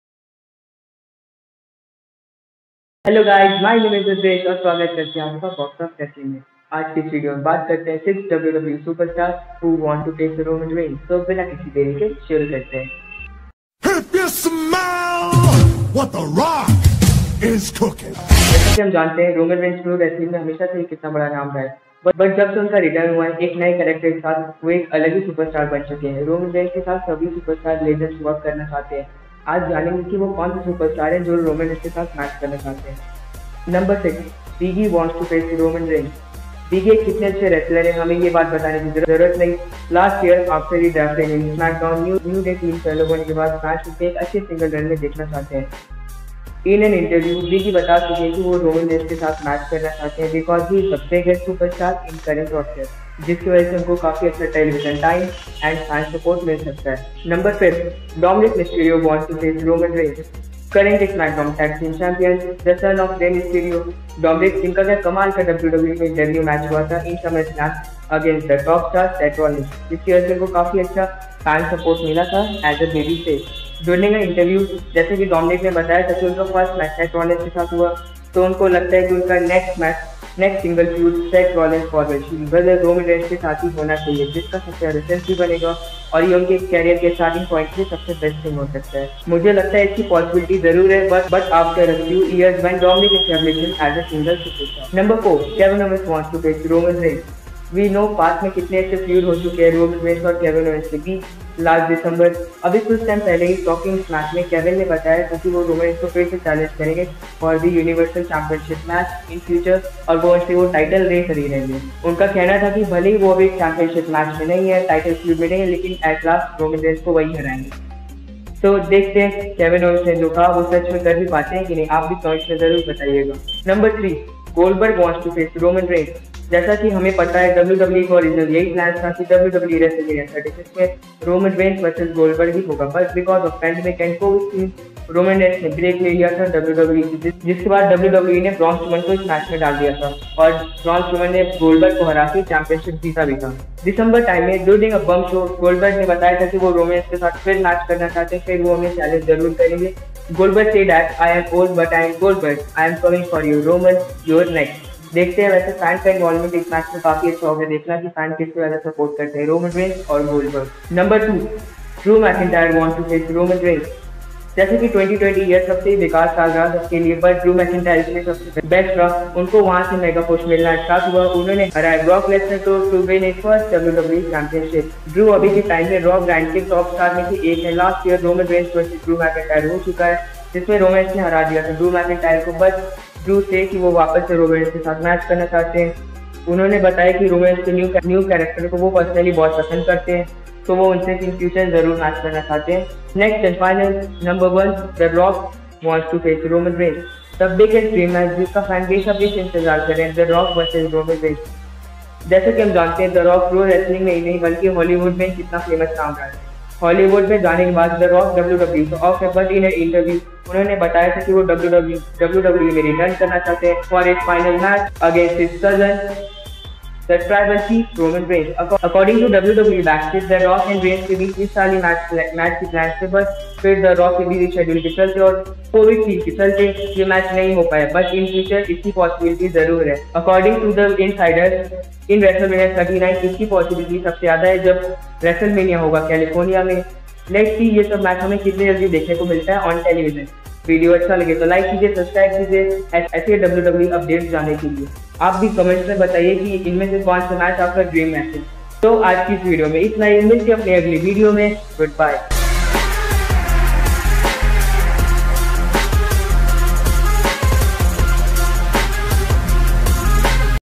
Top 6 Wwe Superstars Who Wants To Face Roman Reigns !!
1)Wwe Superstars Who Wants a Match Against Roman Reigns.
2)Wwe legends Vs Roman Reigns !!
3)Big E Wants a Match Against "The Tribal Chief" Roman Reigns
4) Dominik Mysterio Wants a Single fued against Roman Reigns
5)Kevin oweans Want a Rematch For Universal Championship
6)Goldberg challenge Roman Reigns
7)Drew Mcintyre Wants a fued with Roman Reigns
8)The Rock Wants His Final Match In Wwe Against His Brother.
9)Wwe Wrestlemania 39 The Rock Vs Roman Reigns Plans.

Top 6 Wwe Superstar Who Wants To Face Roman Reigns !!